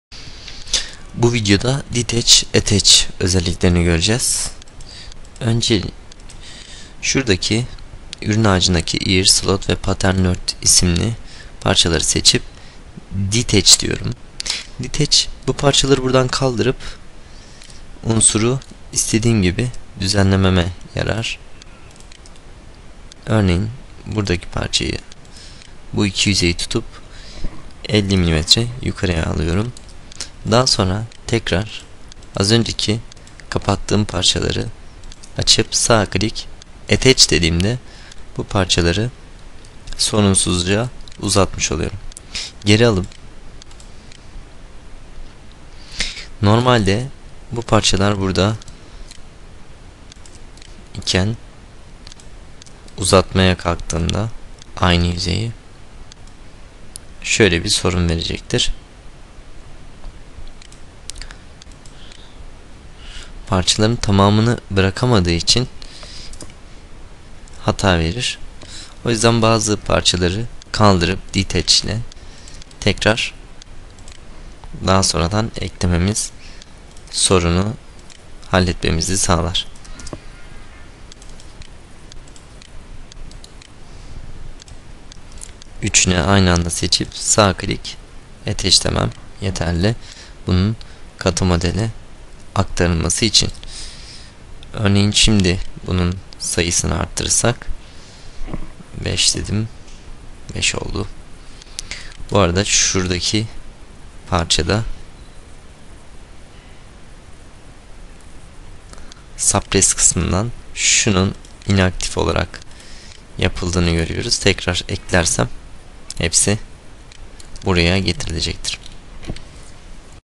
Solid Edge ST6 - Eğitim Videoları (Senkron Modelleme) -59- Senkron Detach Attach
Solid Edge ST6 versiyonunda Senkron modelleme ortamı için hazırlanmış eğitim videoları.